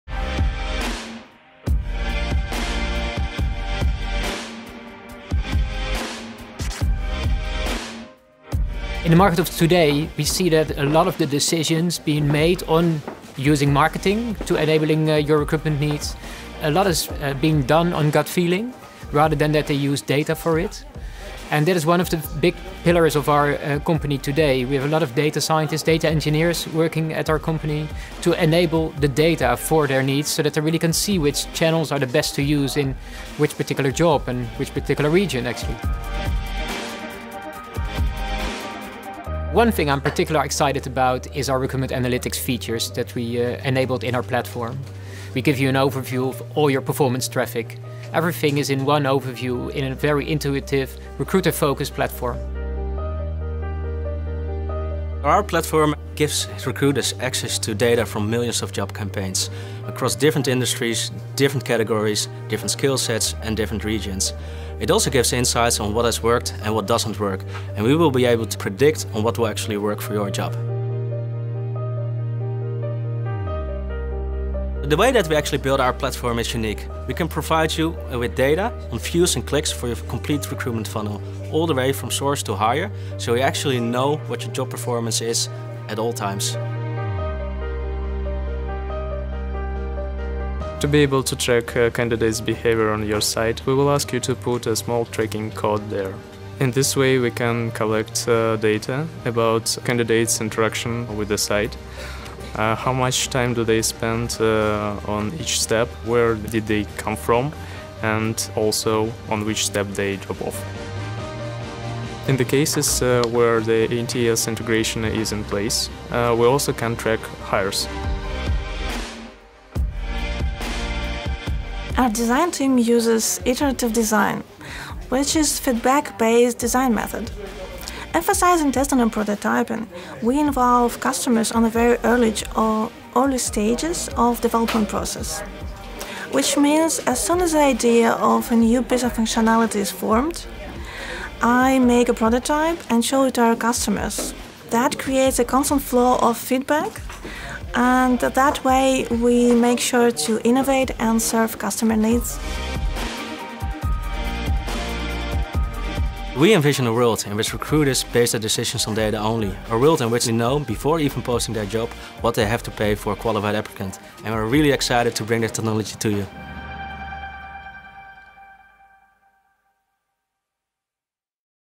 Recruitment Analytics | Krijg volledig inzicht in je Candidate Journey - van source to hire
VONQ's Job Marketing Platform biedt Recruitment Analytics aan om je volledige candidate journey te meten en te analyseren. Gemakkelijk gratis te activeren.